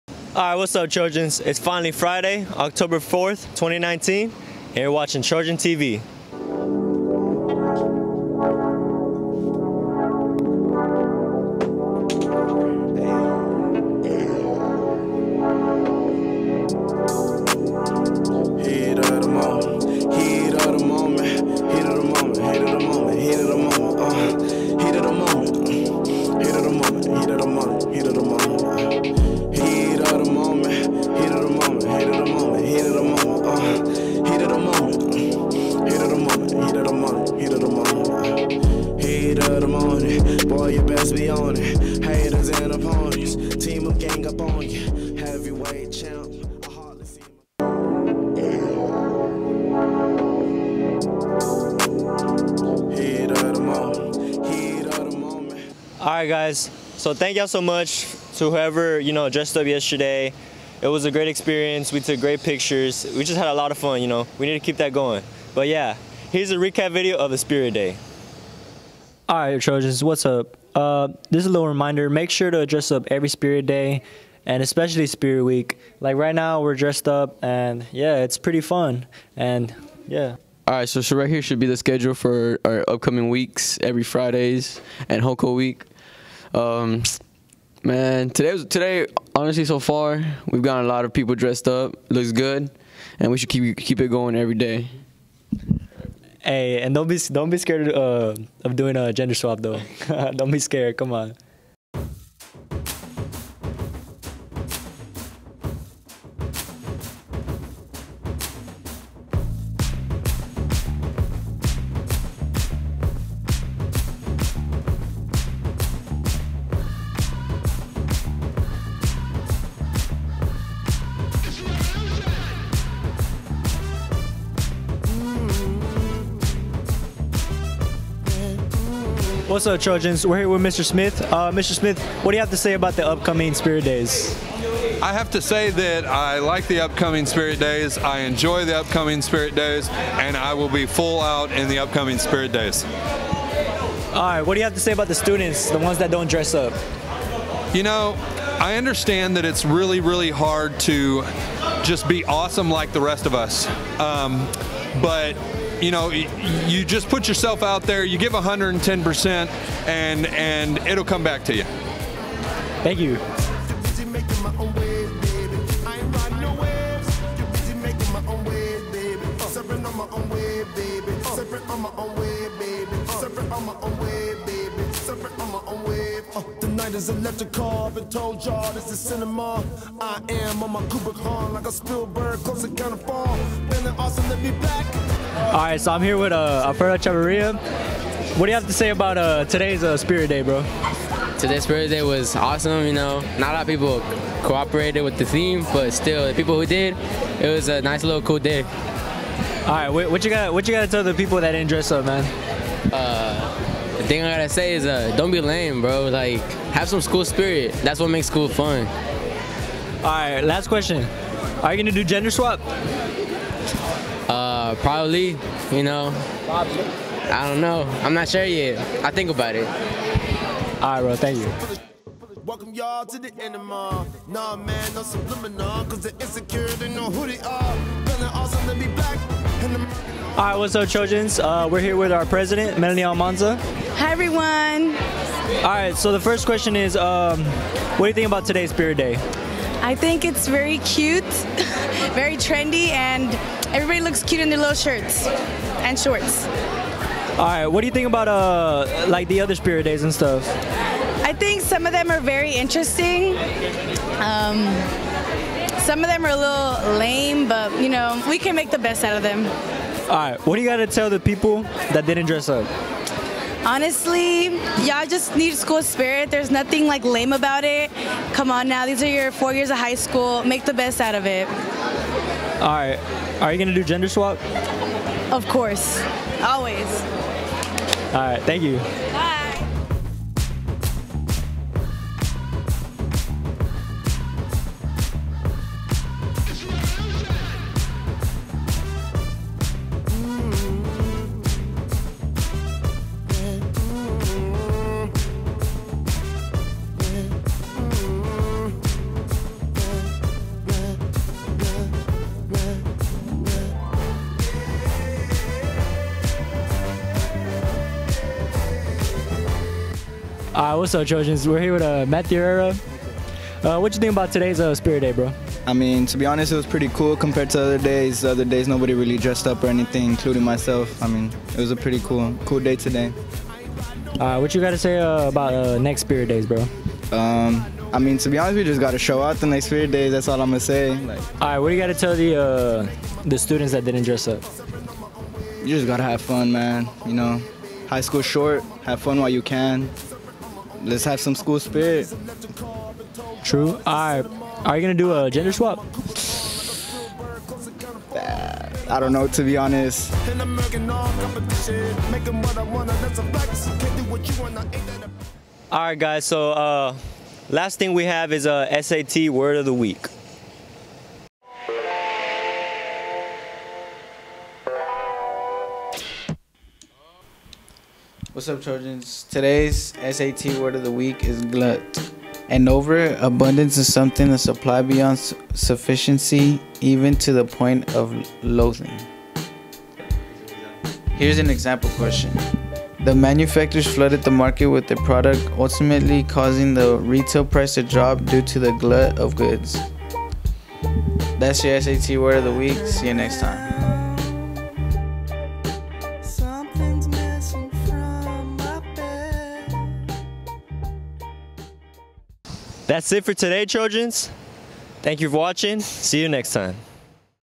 Trojan TV 10.4.2019
Tacky Tourist Spirit Day Highlights, and SAT Word Of The Week